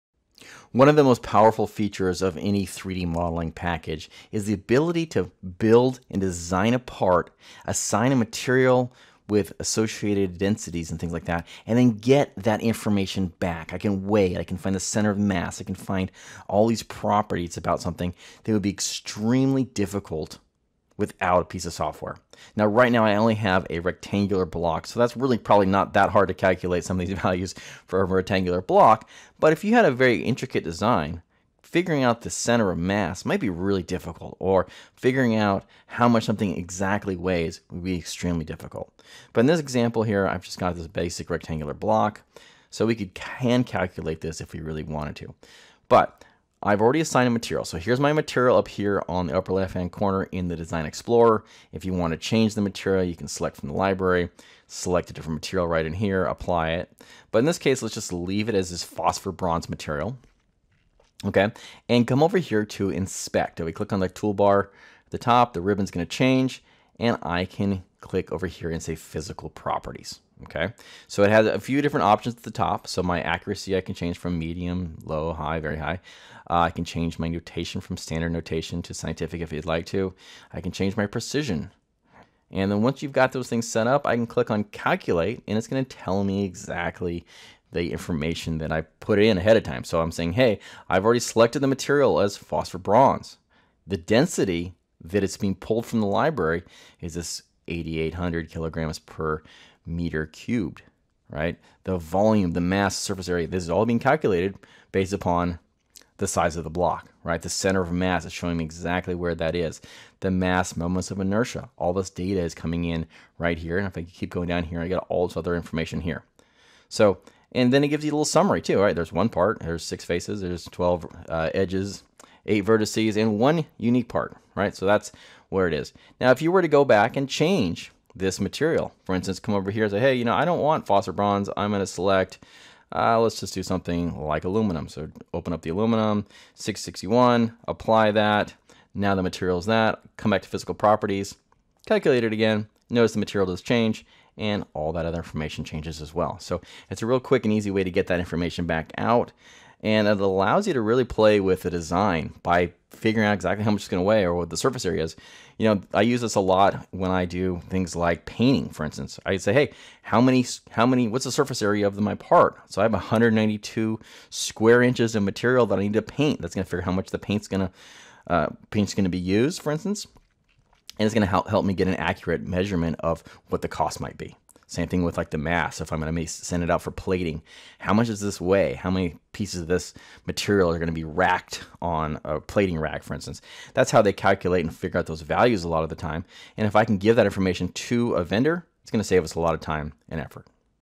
Physical Properties in Alibre Design | 2022 Exercise Series
Learn to measure the physical characteristics of a part like weight and volume.